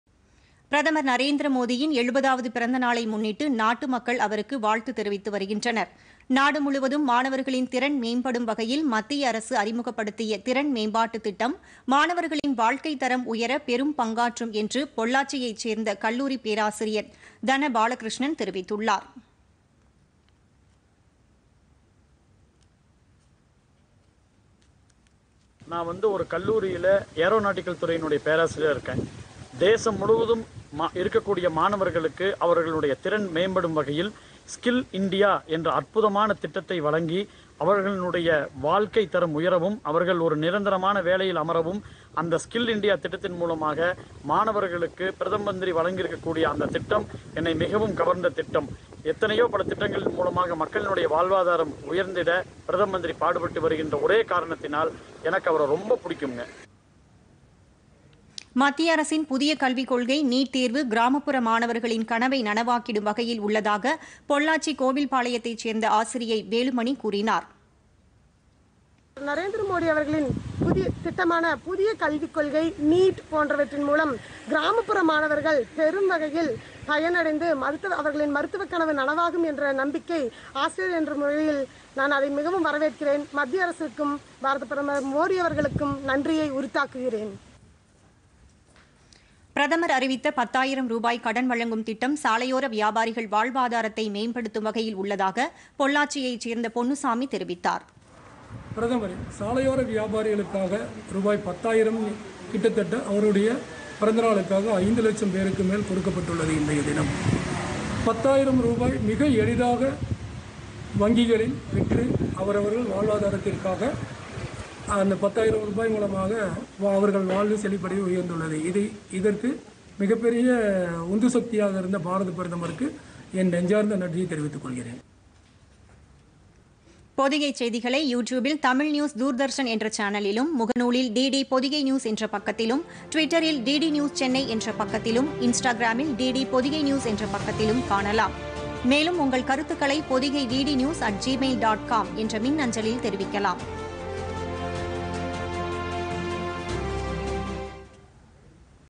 பிரதமர் நரேந்திர மோதி பிறந்தநாள் - மக்கள் வாழ்த்து
பிரதமர் நரேந்திர மோதியின் 70-வது பிறந்தநாளை முன்னிட்டு நாட்டு மக்கள் அவருக்கு வாழ்த்து தெரிவித்து வருகின்றனர்.